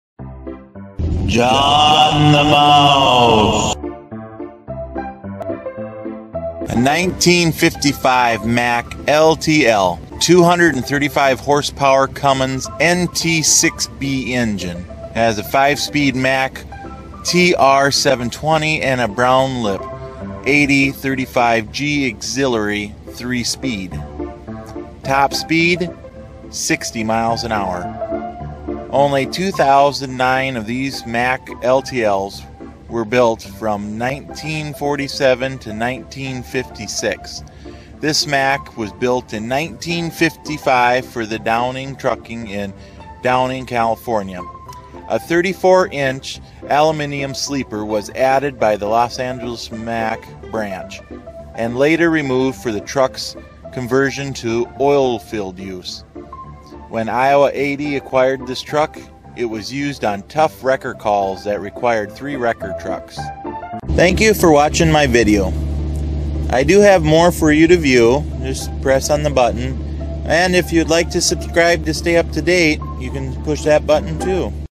1955 Mack LTL in 4K
Only 2,009 of these Mack LTL’s were built from 1947 to 1956. This Mack was built in 1955 for Downey Trucking in Downey, California. A 34” aluminum sleeper was added by the Los Angeles Mack branch, and later removed for the truck’s conversion to oil field use. When Iowa 80 acquired this truck, it was used on tough wrecker calls that required three wrecker trucks. Top speed—60 MPH

Engine Type:
235 hp Cummins NT 6 B Engine

Transmission Type:
5 speed Mack TR720 and Brown Lipe 8035 G Auxiliary 3 speed

Thank-you for all your comments and viewing the video. I have other videos on my YouTube page.

cloakcoin:CBs9HooP6g2u1y4pFJdWdaiaeBV1b7x8Xe